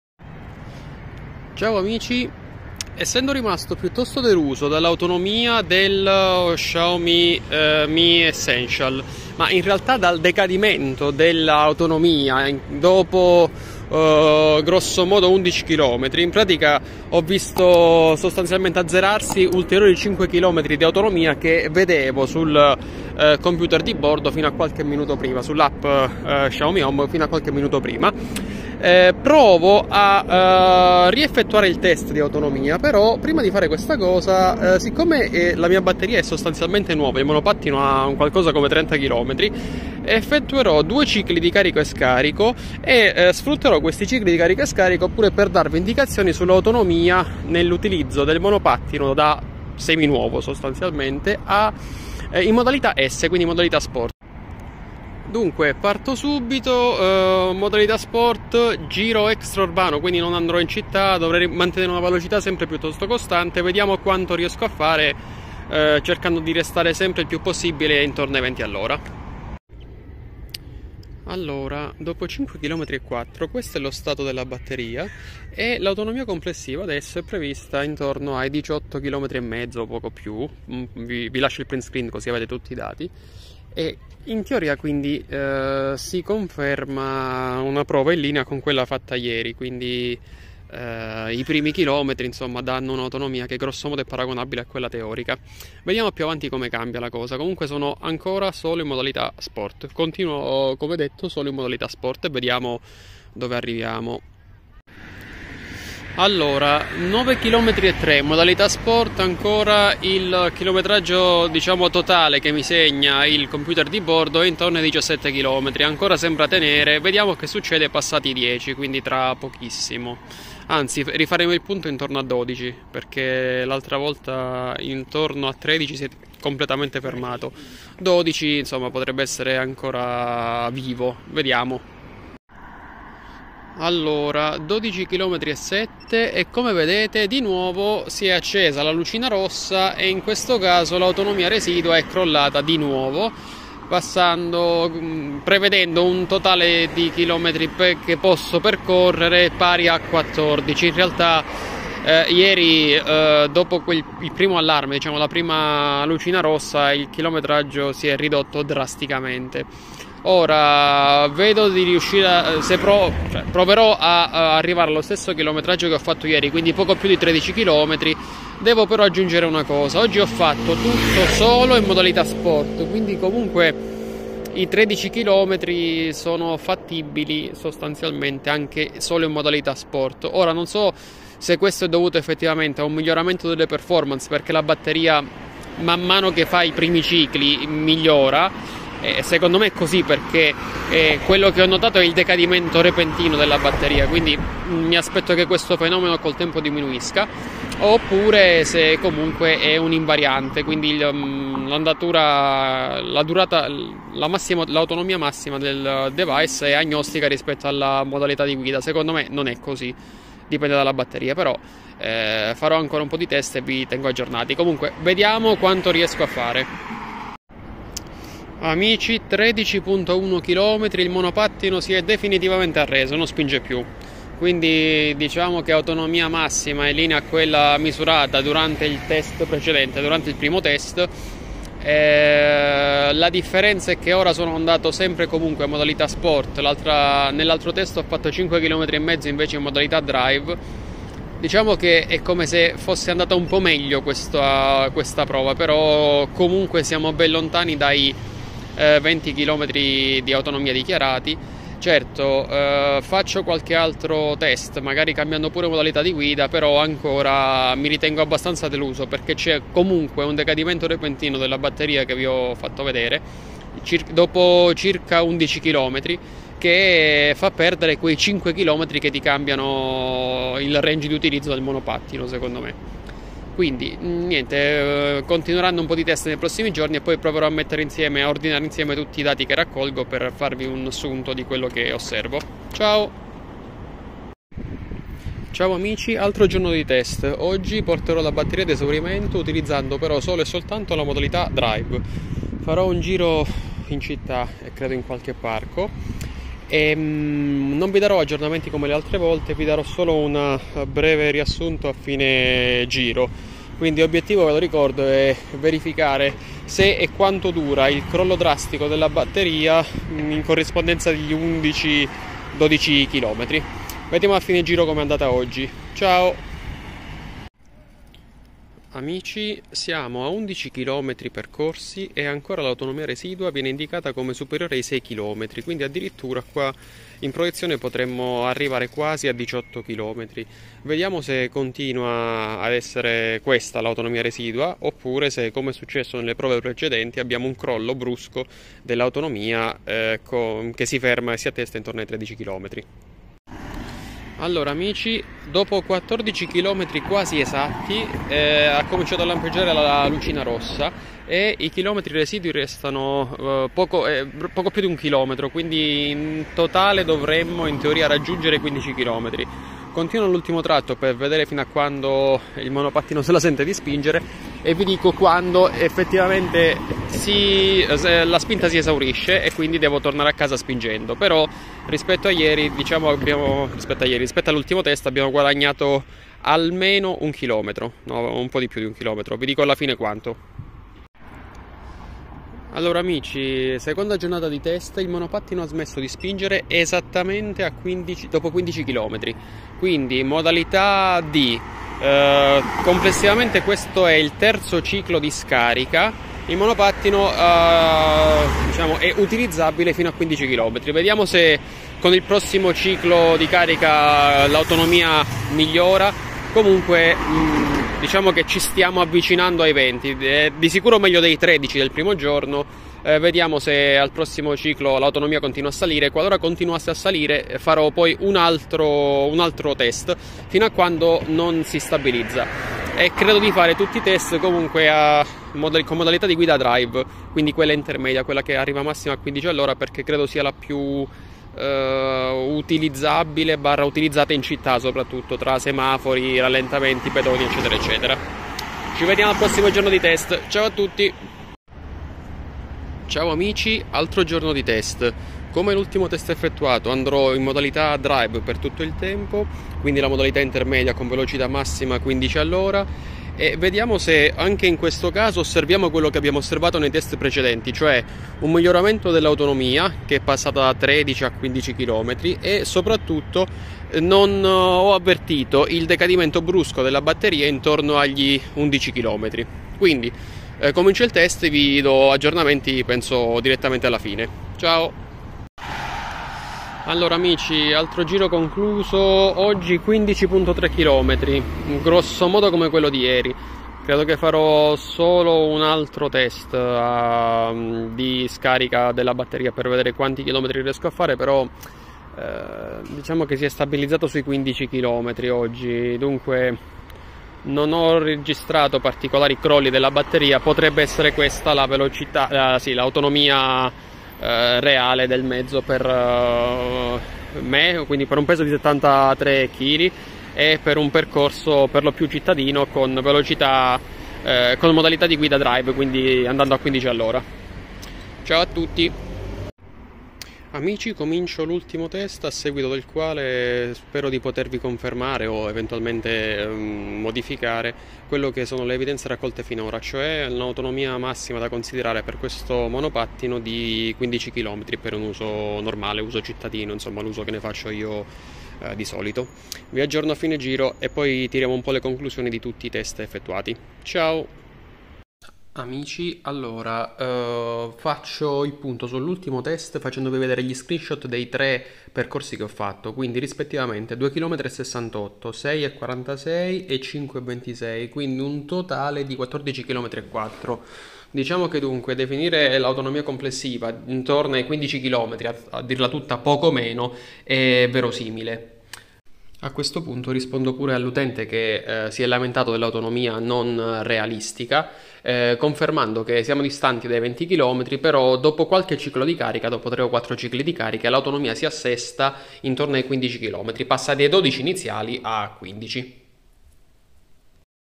Autonomia Xiaomi Mi Essential dopo 4 cicli di carica e su percorsi cittadini
A seguito della segnalazione sulla scarsa autonomia dello Xiaomi Mi Essential, ho provato ad effettuare un po' di test di autonomia dopo aver fatto completare 4 cicli di carica e scarica alla batteria. Obiettivo era verificare se i cali repentini dell'autonomia, registrati in occasione dei primissimi test, potessero semplicemente essere un problema passeggero dovuto alla batteria "troppo nuova" oppure se siamo in presenza di un comportamento "consolidato" del monopattino e che è quindi da considerarsi un difetto.